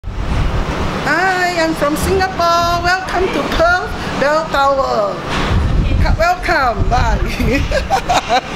The Bell Tower Video Postcard - Christina Wee
Christina Wee from Singapore want to say hello from the Bell Tower observation deck.